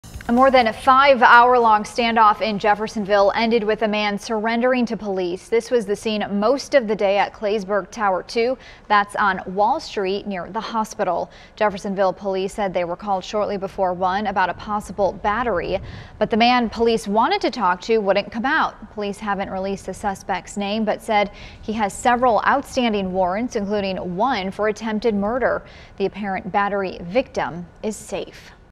Man wanted for attempted murder arrested after 5-hour standoff with Jeffersonville police, SWAT
According to Maj. Isaac Parker, the standoff ended around 6:15 p.m.